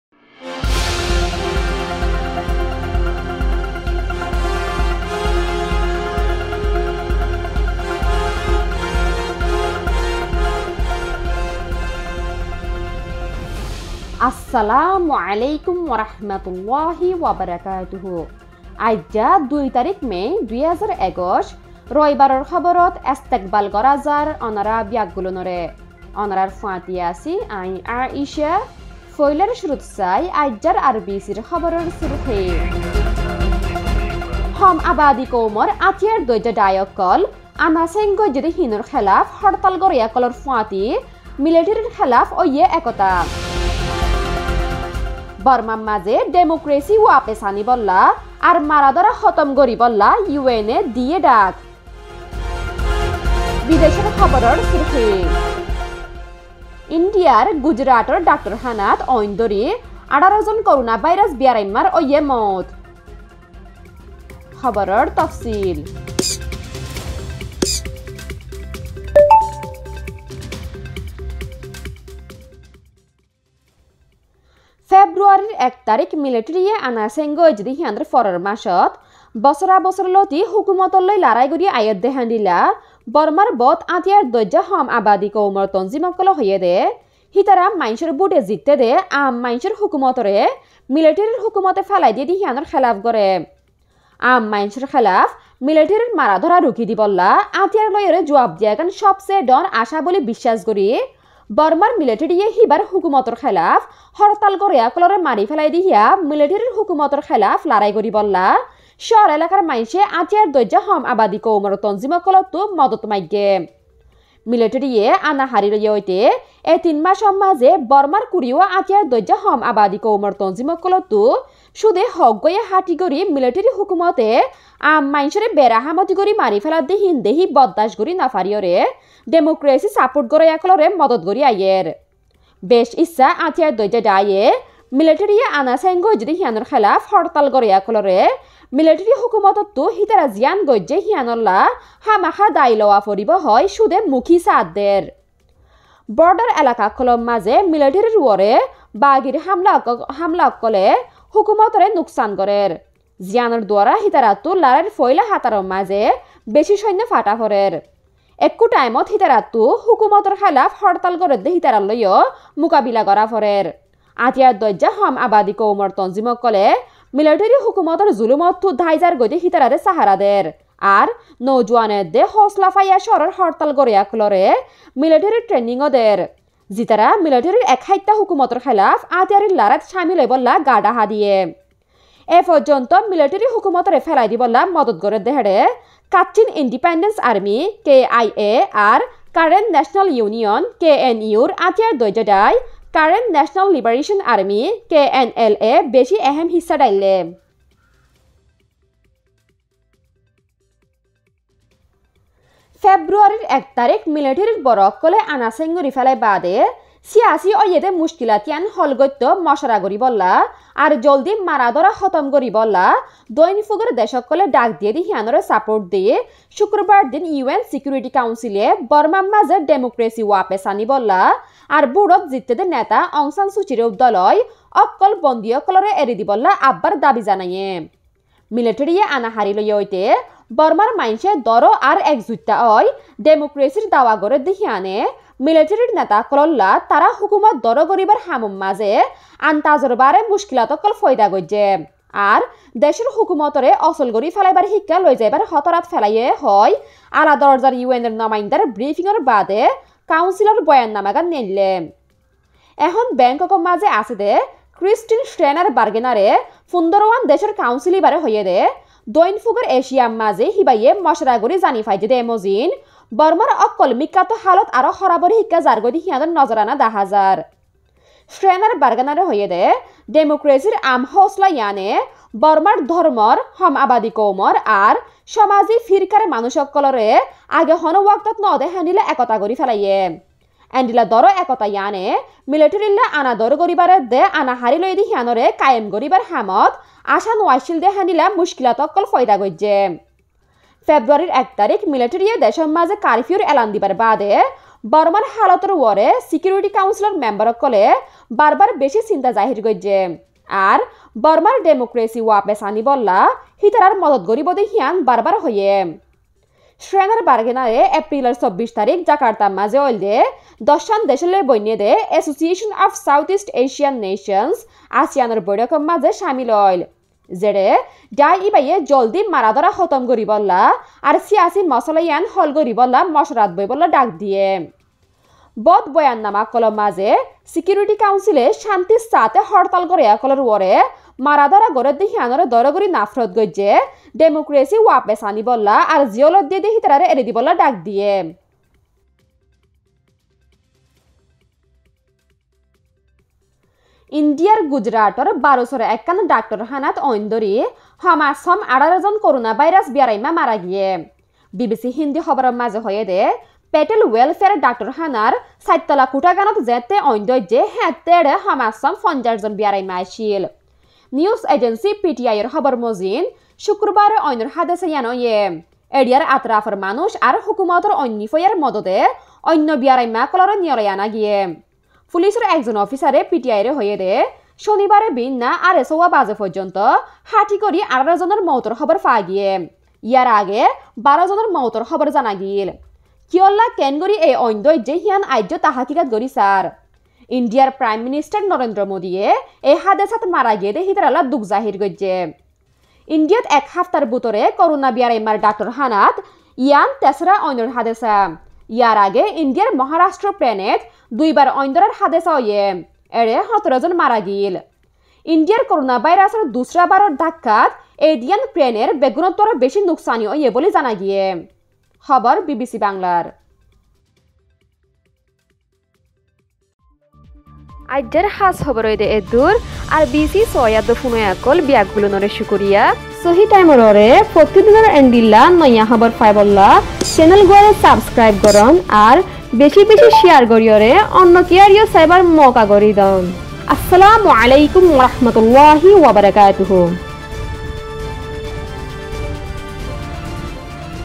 Rohingya Daily News 02/05/2021 - RBC News
RBC news Headlines:
1.Ethnic Armed Groups Unite With Anti-Coup Protesters Against Myanmar Junta.
2. 2.UN calls for restoration of democracy in Myanmar, end to violence.
3. 3.Gujarat: 18 Covid patients die in fire incident at Bharuch Welfare Hospital.